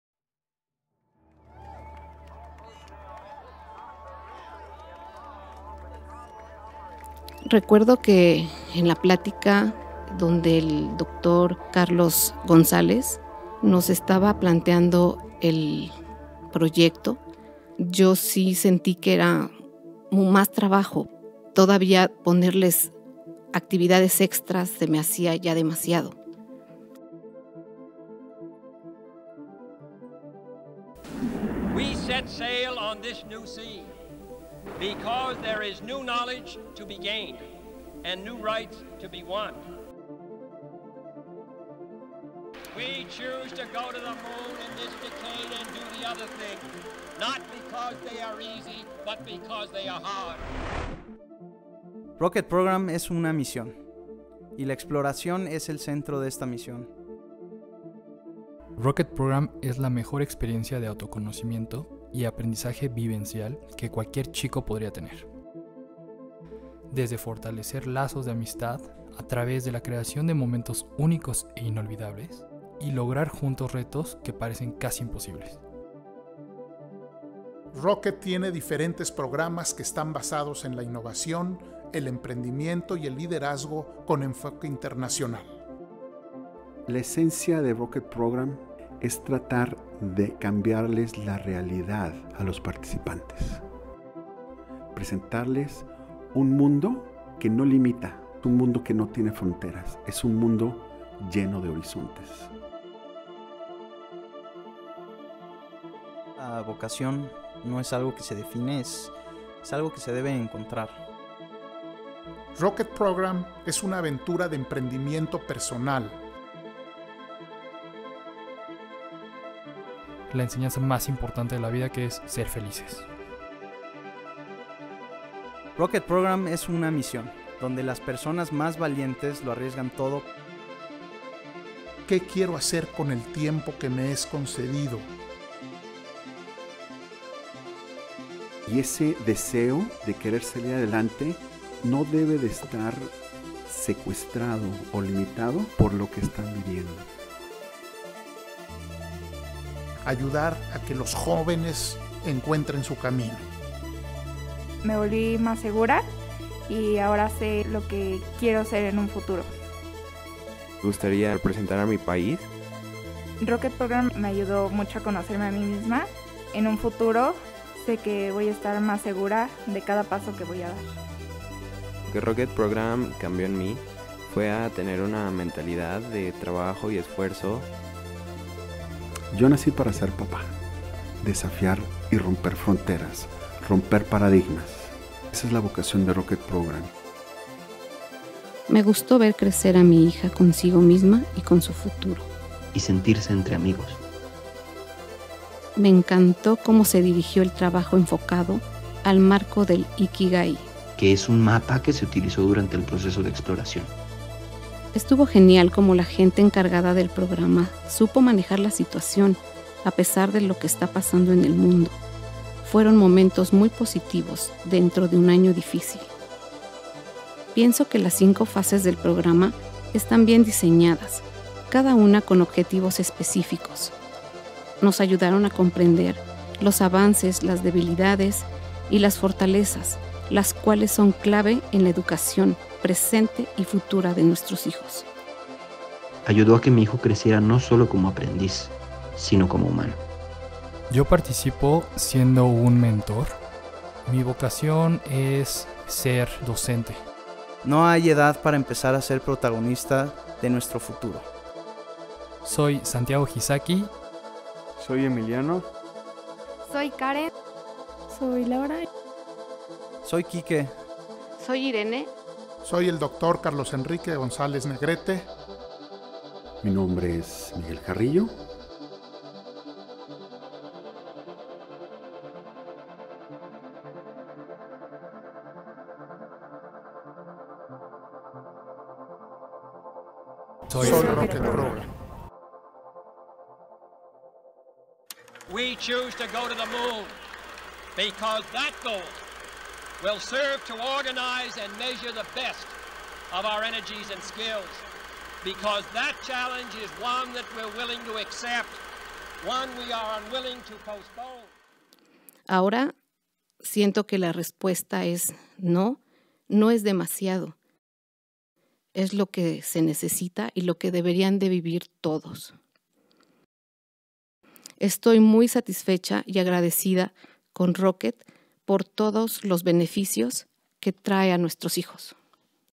Rocket Program
Que es el programa de emprendimiento de Rocket Program en colaboración con florida international University